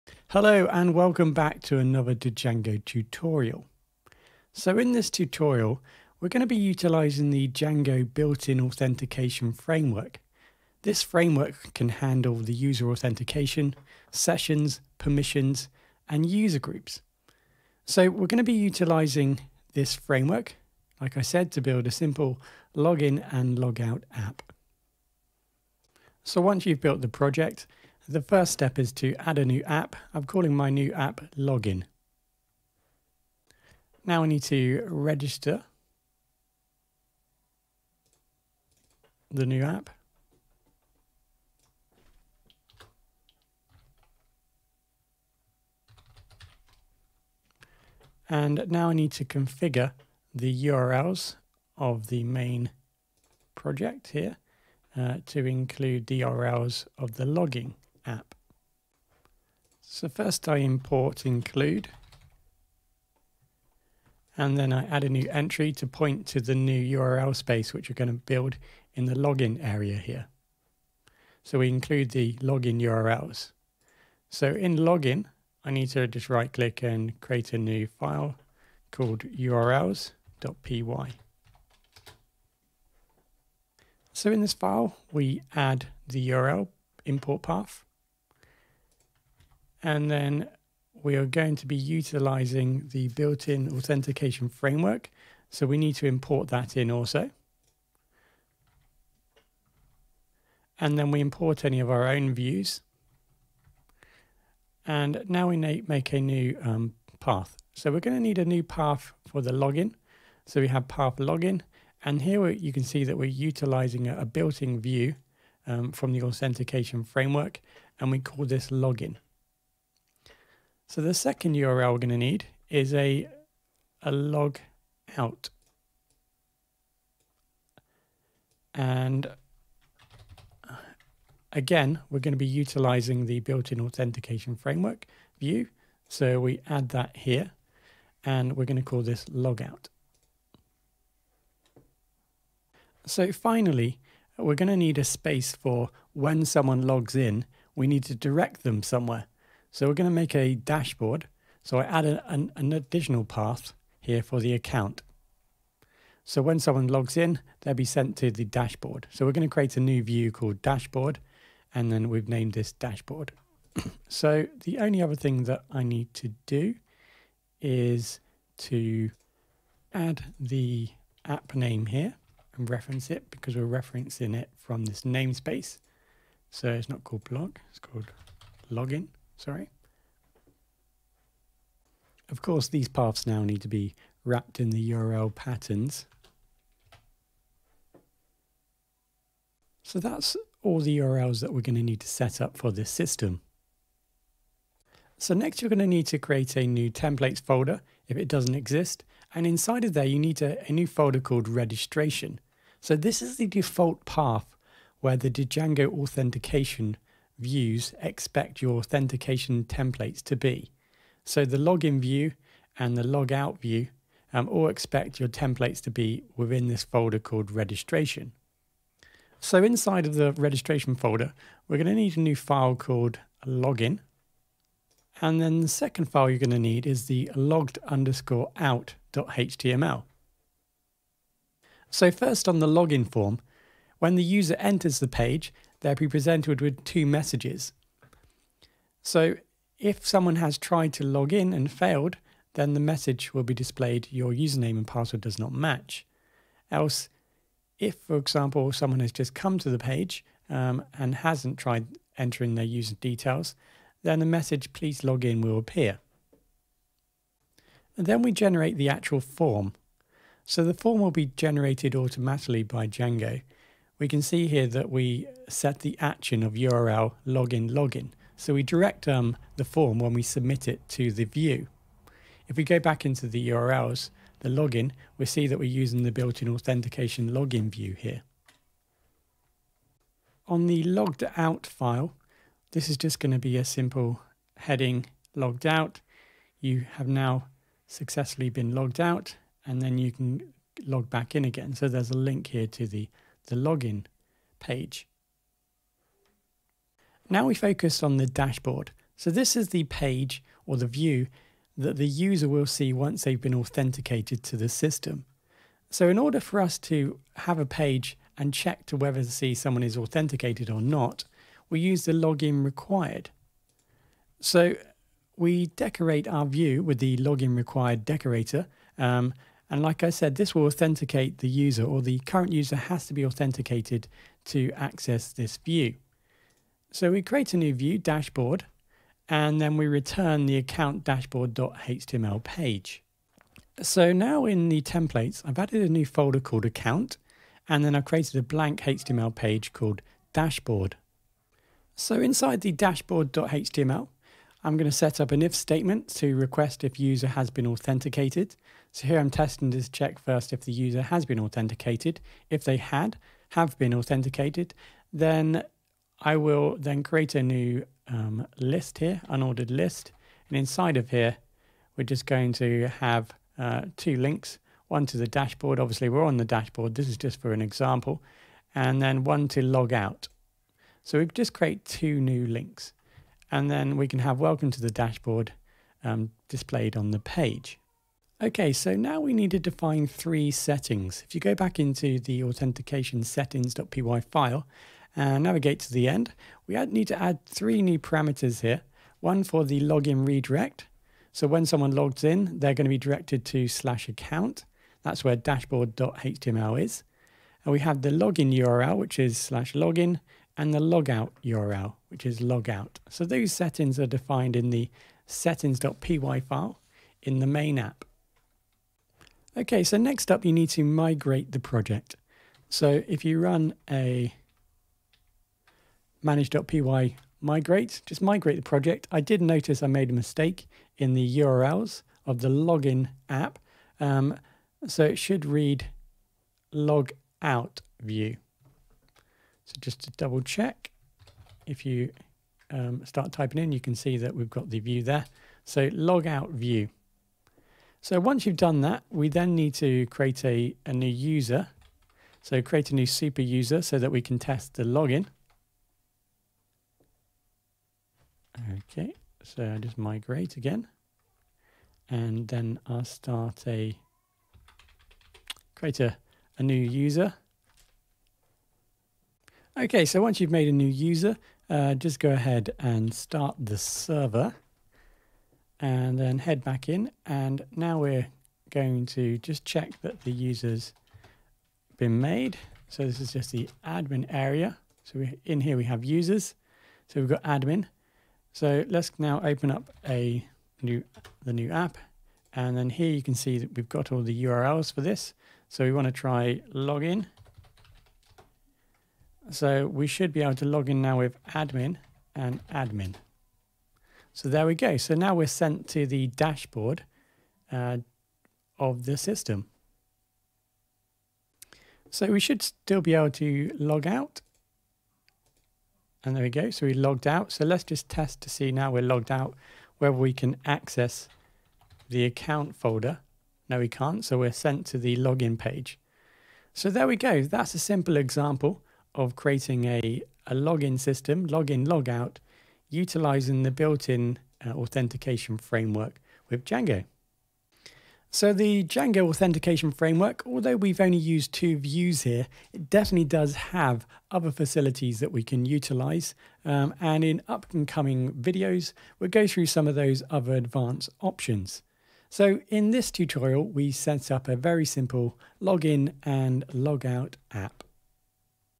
Django User Login Authentication Tutorial - Log in and log out with Django Authentication Framework
A Django user login system is demonstrated using the Django Authentication Framework